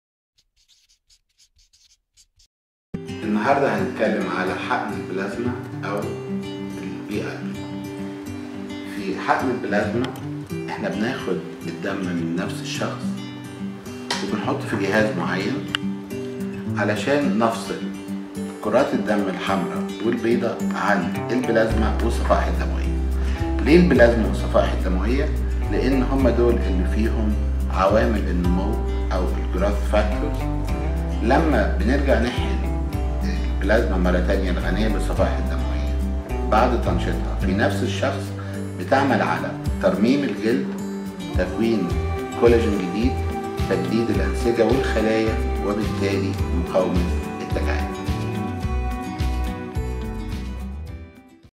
PRP General Info
Platelet Rich Plasma is a 100% natural method that uses the patient’s own blood components to stimulate the renewal of damaged tissue in areas of the face, neck, décolleté, hands and body.

Extracted blood platelets that contain proteins, nutrients, and a variety of growth factors are injected in the area to be treated. When growth factors are activated by injection, repair of damaged tissue begins, generating collagen and hyaluronic production.

A natural enhancement of the skin’s appearance begins to take place and youthfulness, suppleness and volume are restored to the treated areas.